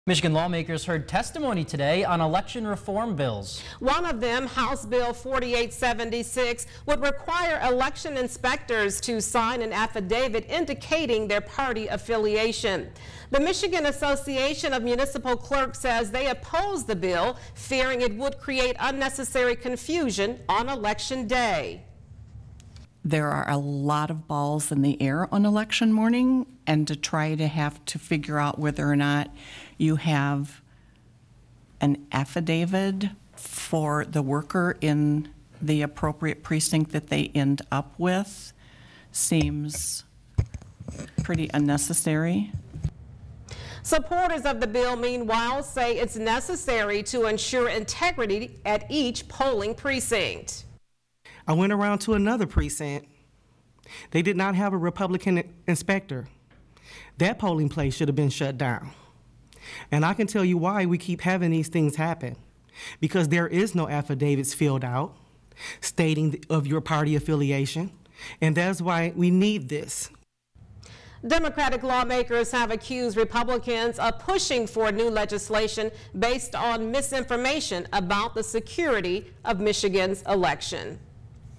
HB 4876
The Michigan House Committee on Elections and Ethics listened to testimony today on several election reform bills.